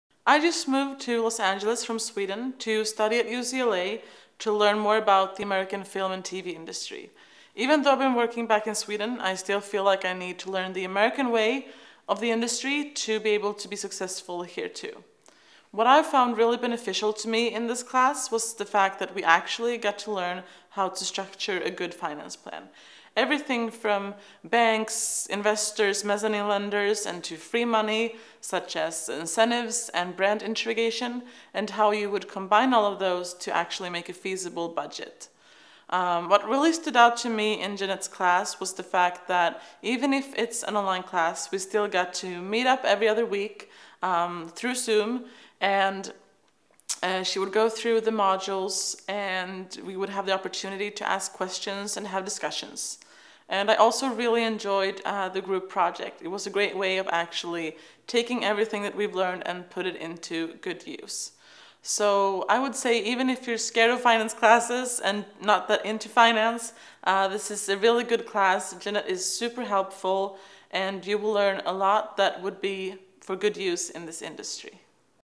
"Entertainment Finance" class testimonial from Josefin, Swedish Producer
Swedish Producer Josefin shares her view on Jeanette Milio's entertainment finance class.